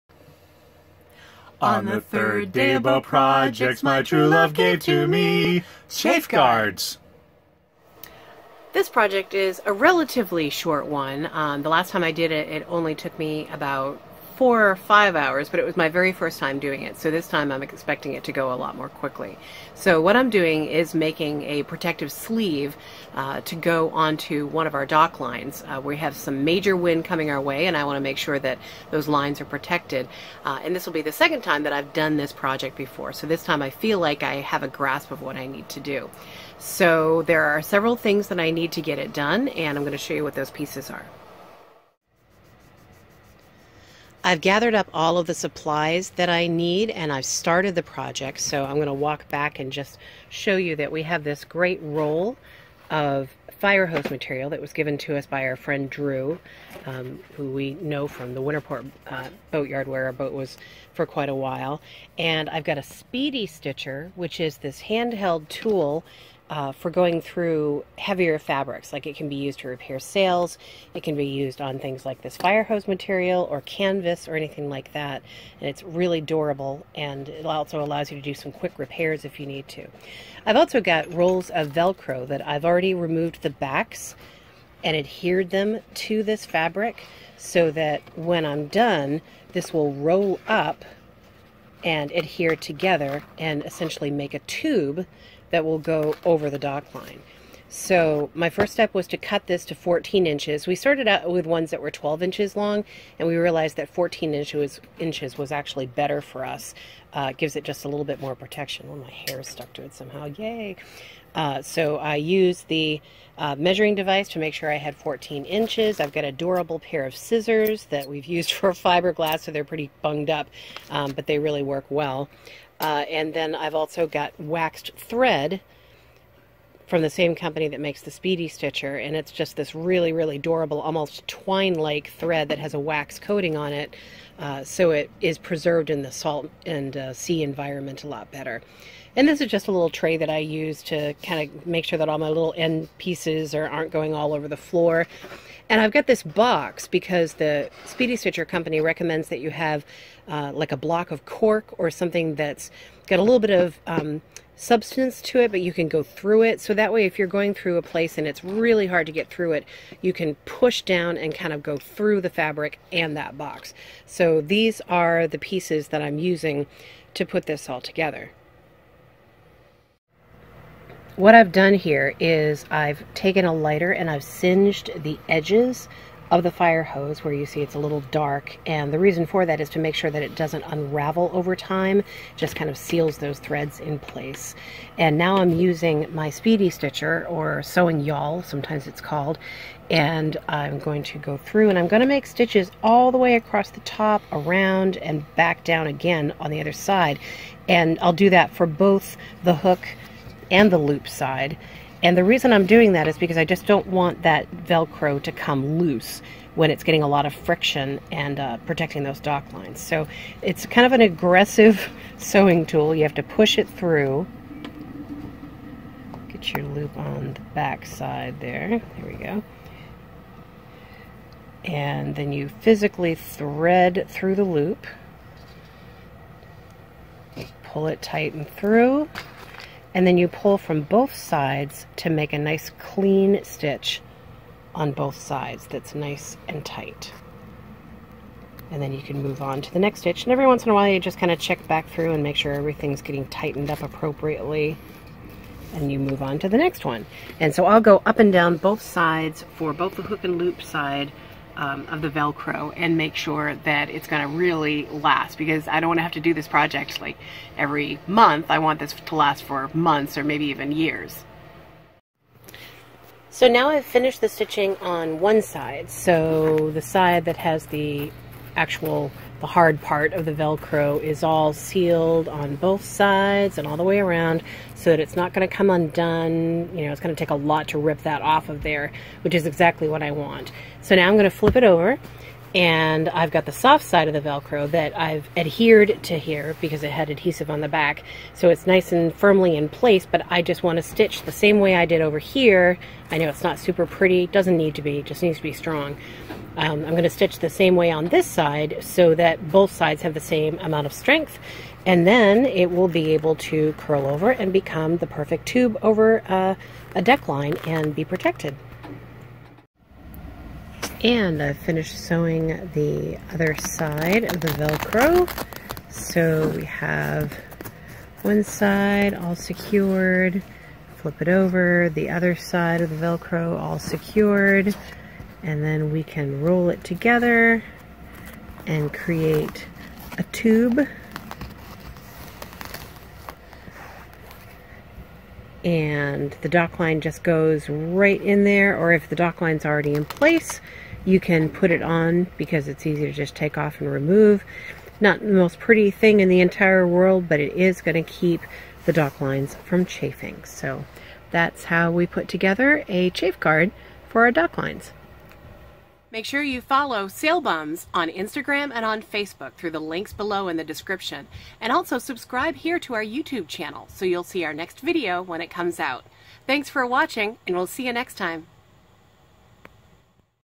3rd Day of Holiday Boat Projects: Chafe Guards
The Sail Bums are taking on 12 days of holiday boat projects as we transition from 2020 to 2021 in Newport, RI. Day 3: chafe guards for dock lines.